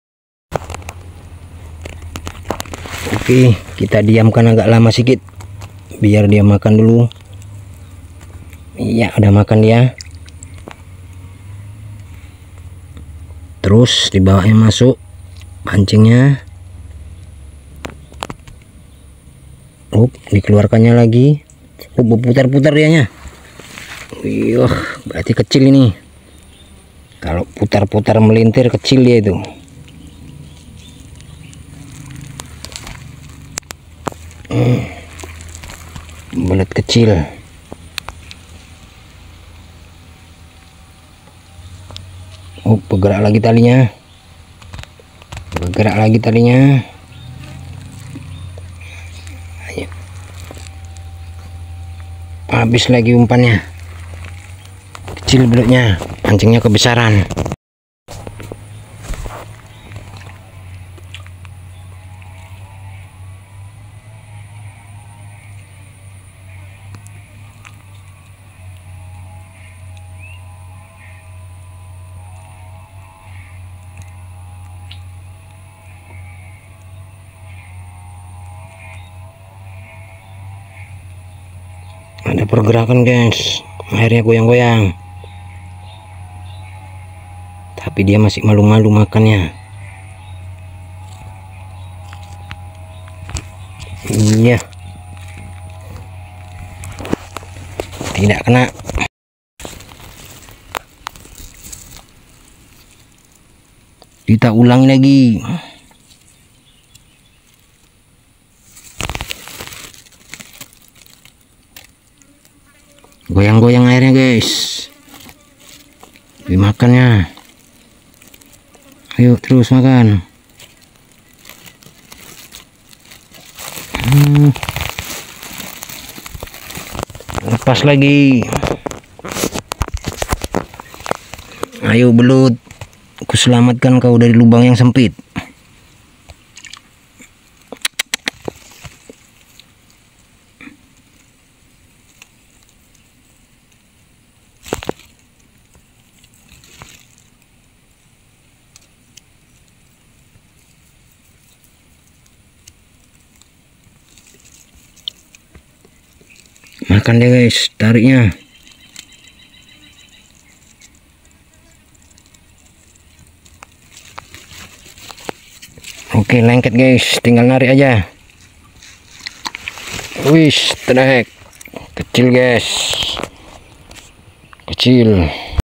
baru belajar mancing belut sawit, mencari 1 ekor lama juga
mancing belut di kebun sawit